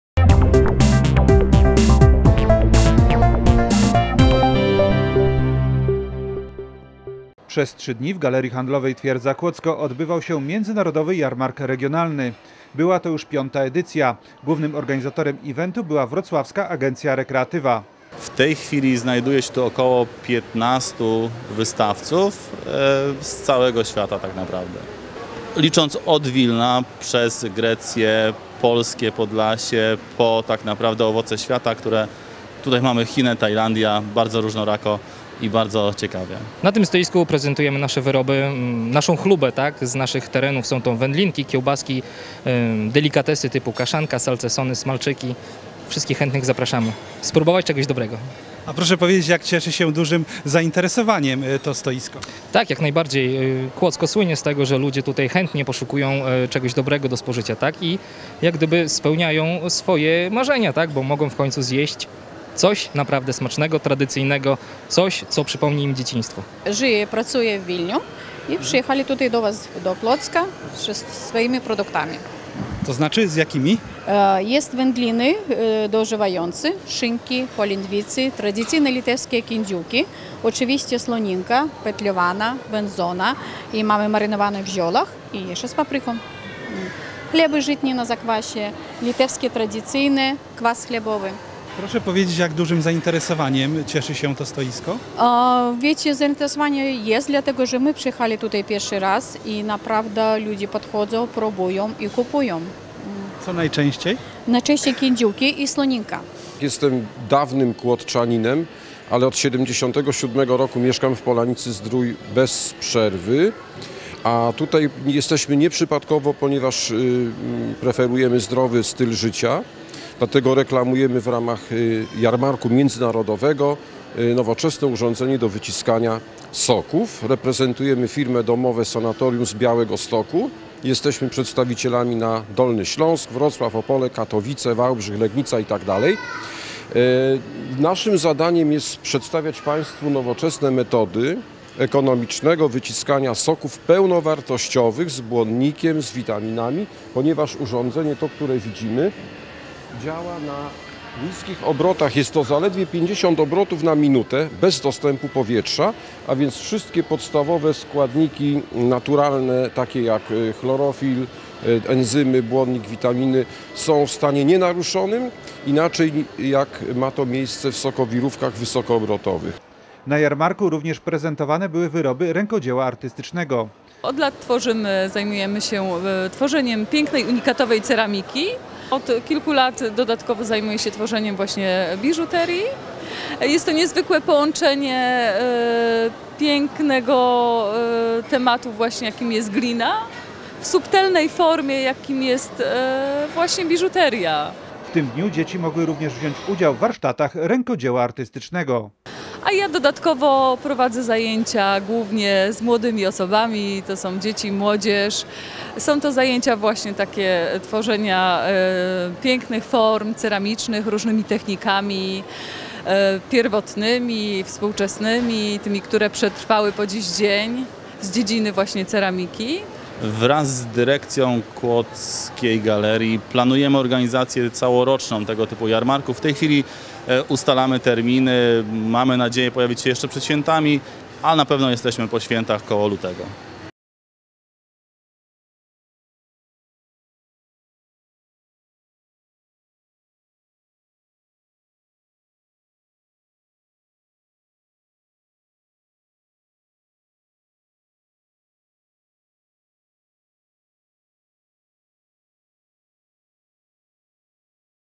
Międzynarodowy Jarmark Regionalny
W Galerii Handlowej Twierdza Kłodzko odbył się Międzynarodowy Jarmark Regionalny.